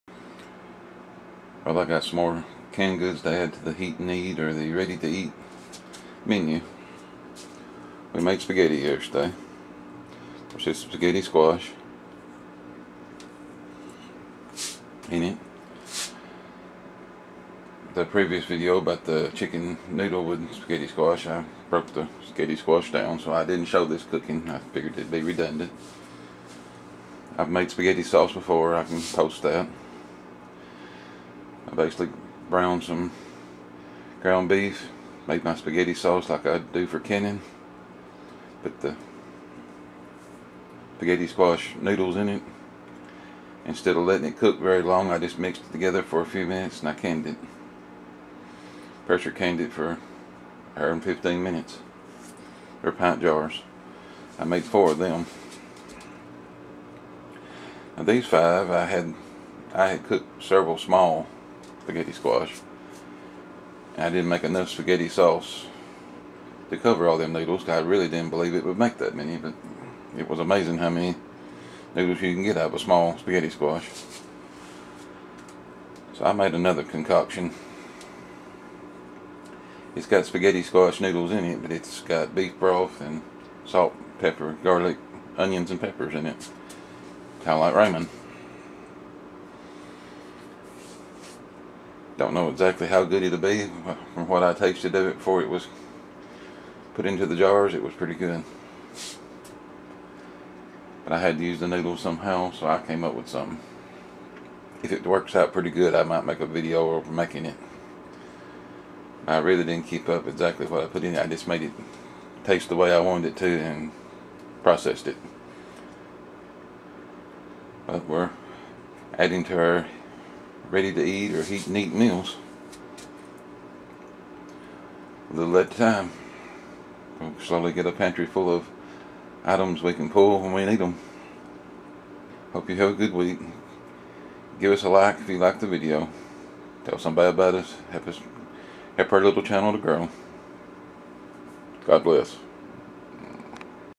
Ready to Eat Canned Spaghetti & A little Different Noodle Meal...
This gives us a few more choices! Sorry I didn't Realize I have never made a video over homemade Spaghetti Sauce...
Spaghetti
2lb Ground Beef, Salt, Pepper, Italian seasoning,Cooked but lightly Browned
The Sauce was 5 quarts tomato juice cooked down till thick,  About a Onion, 1Bell Pepper, Salt, Garlic, Italian seasoning, sugar...